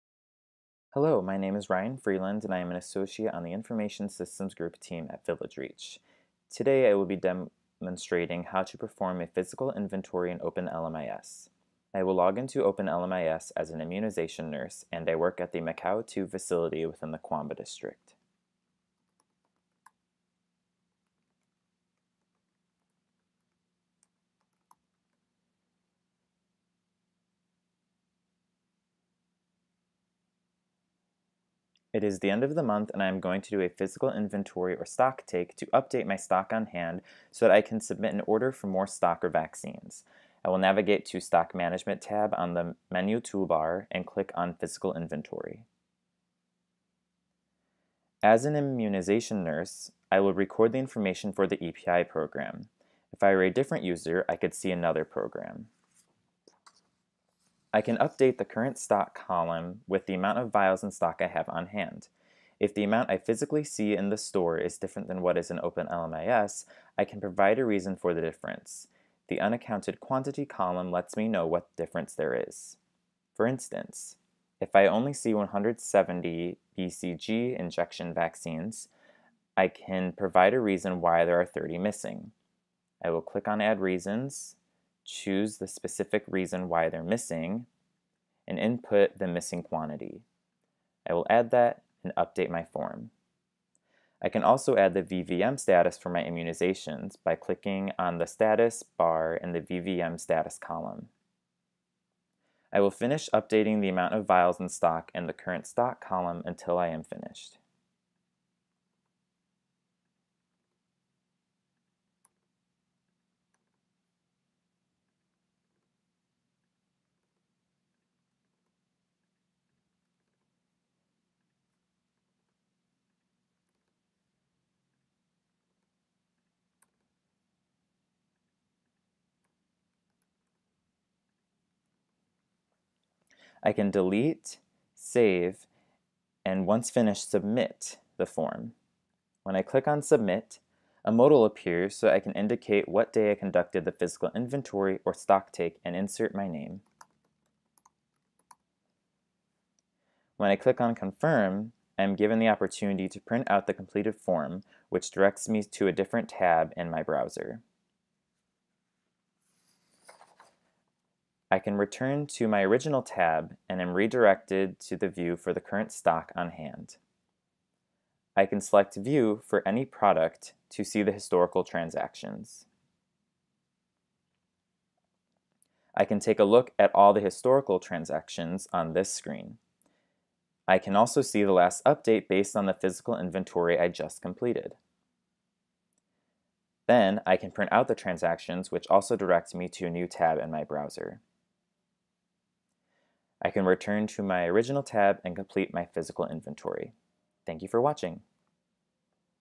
OpenLMIS Version 3.3 -- Physical Inventory
A demonstration video on how to perform a facility's physical inventory in OpenLMIS version 3.3.

This video lies in the "OpenLMIS v3.3 Demo Workflow" playlist to demonstrate the workflow across different OpenLMIS end users.